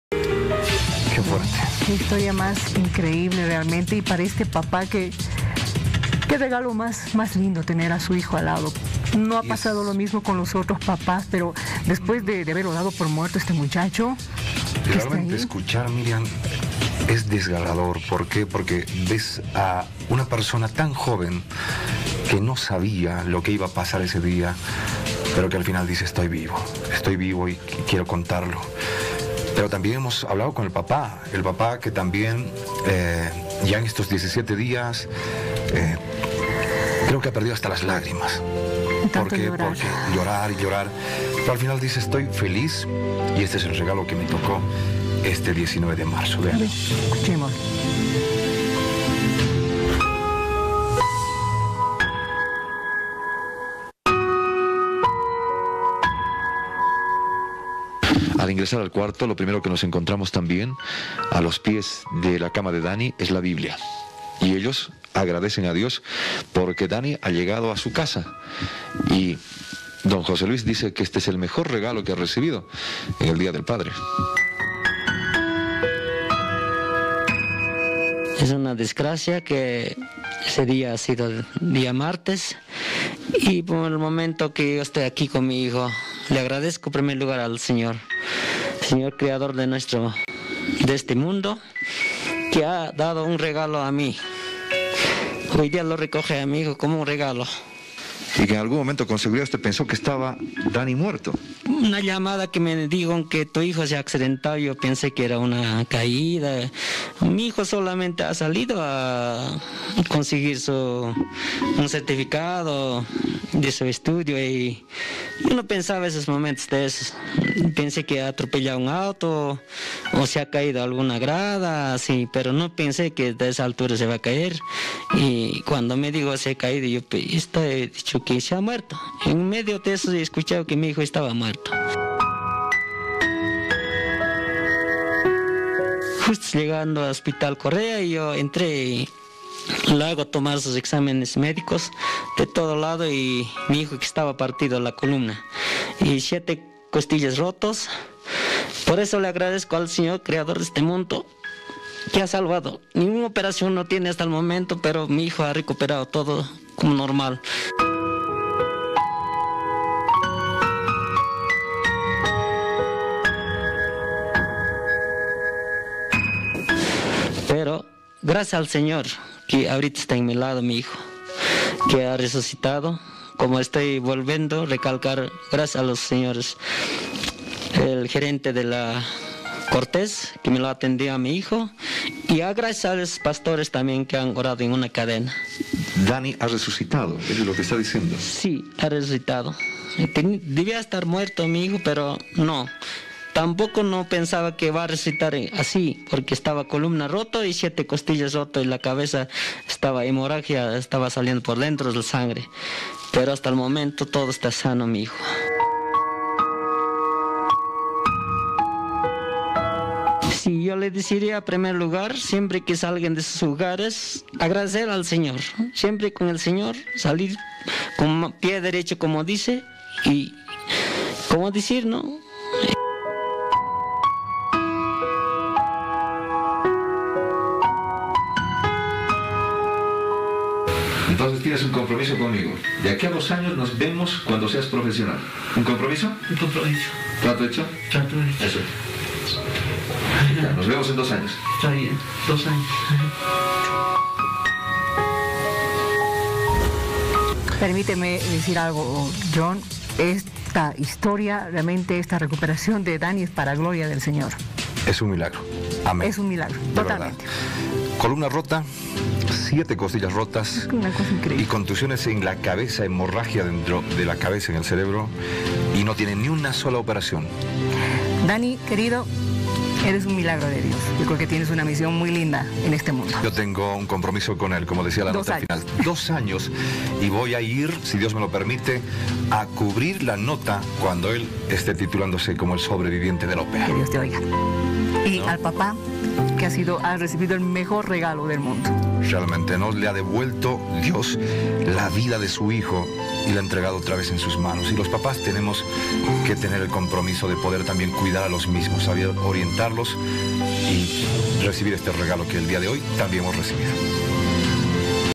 Dieron por muerto a su hijo, hoy su vida es el mayor regalo
Hoy vamos a conocer a Danny, un estudiante que de milagro está vivo y nos relata lo que vivió cayó, de un último piso en la UPEA.